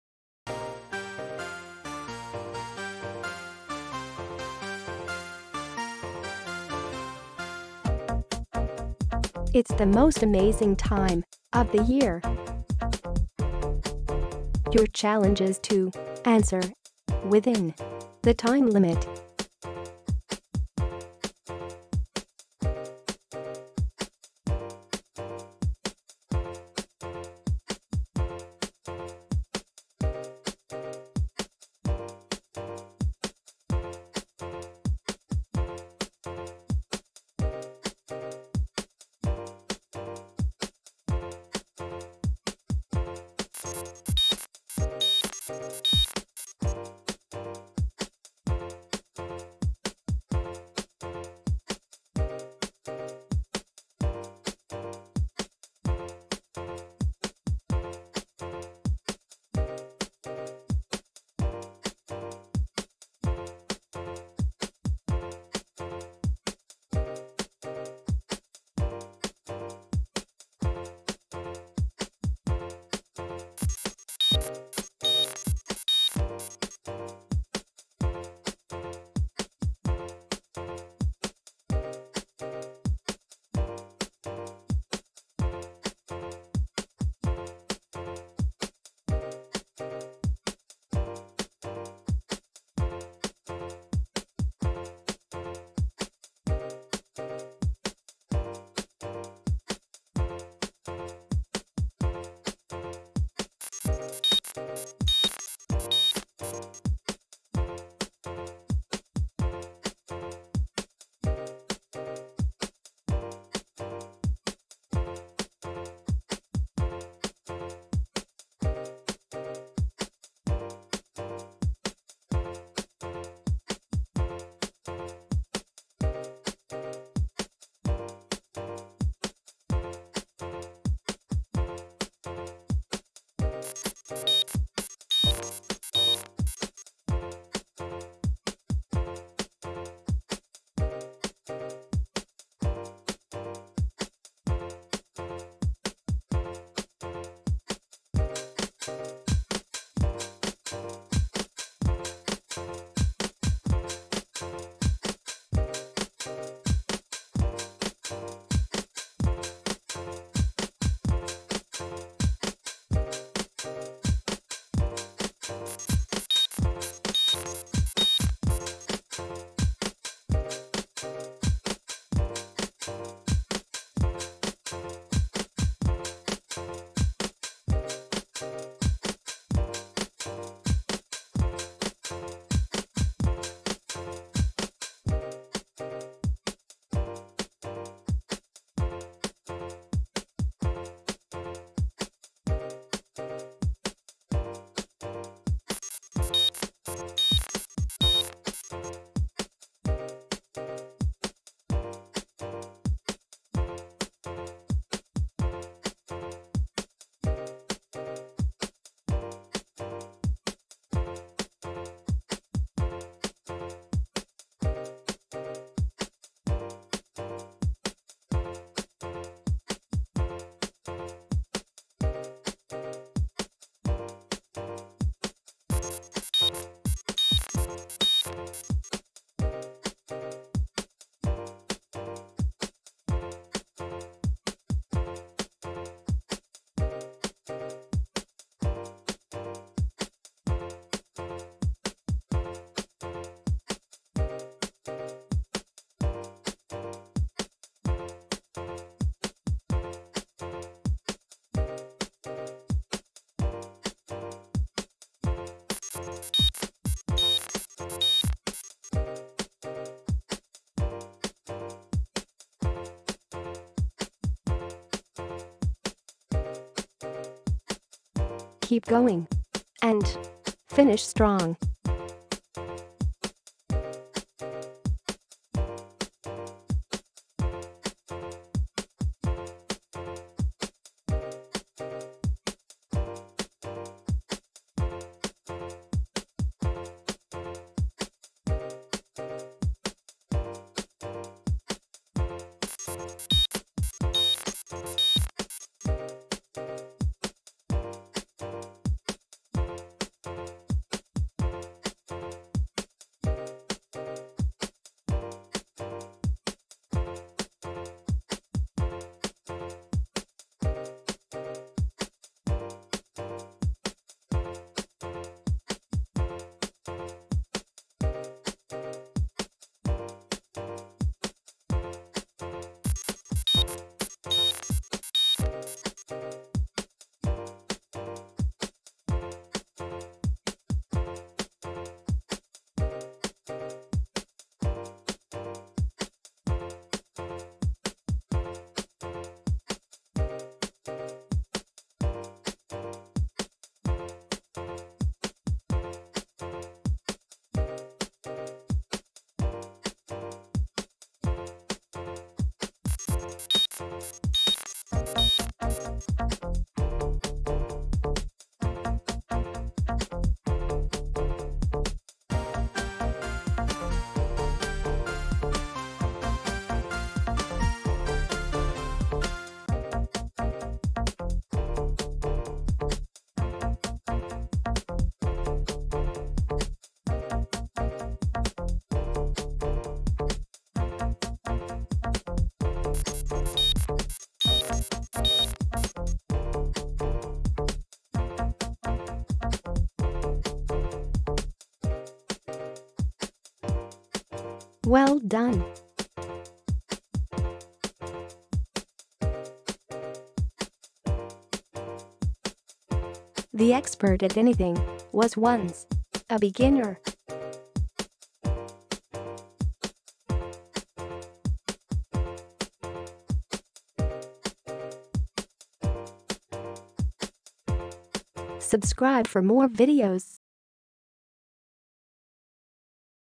Christmas Themed Math Lesson | Winter Math | Multiplication Facts for Kids
A little progress each day adds to BIG results!

Remember to practice daily. A little practice each day will help you memorize your multiplication tables sooner.